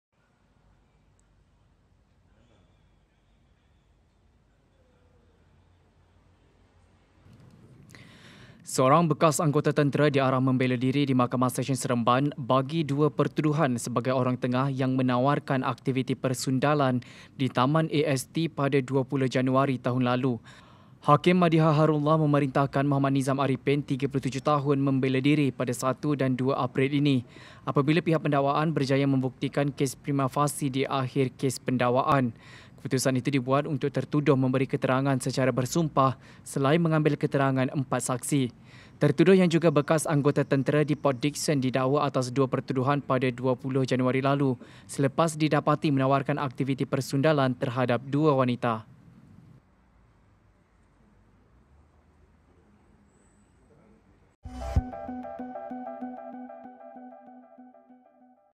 Bekas tentera diarah bela diri
Seorang bekas anggota tentera diarah membela diri di Mahkamah Sesyen Seremban, bagi dua pertuduhan sebagai orang tengah yang menawarkan aktiviti persundalan di Taman AST pada 20 Januari tahun lalu.